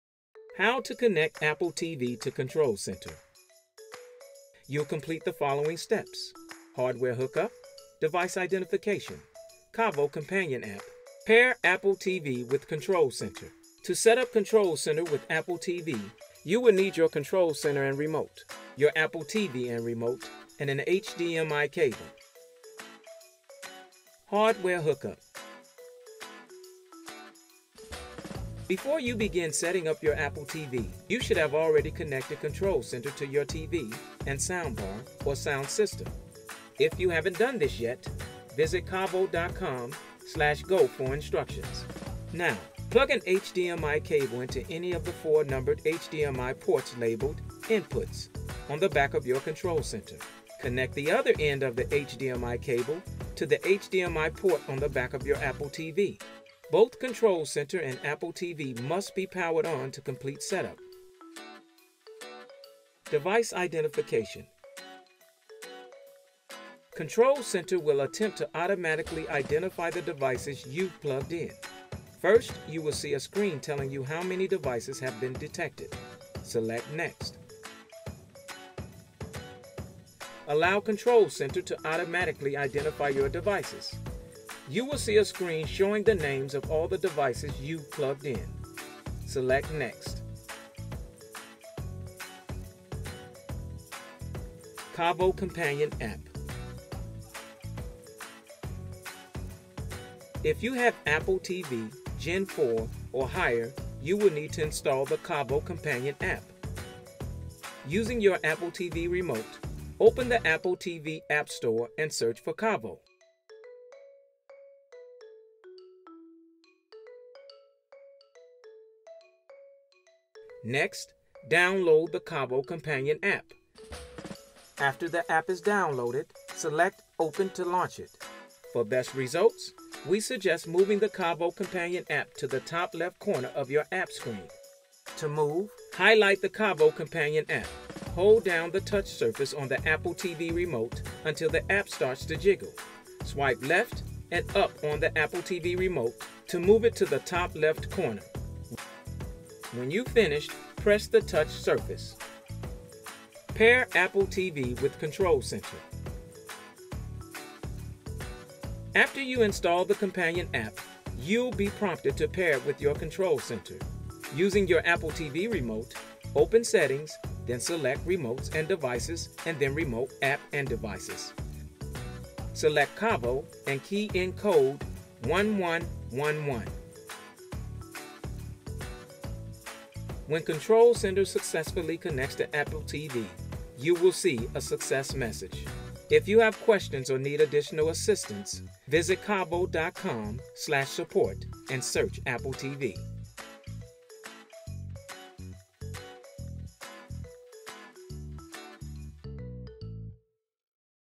How To Connect an Apple TV to Control Center
In this video you'll learn how to set up your Apple TV with Control Center.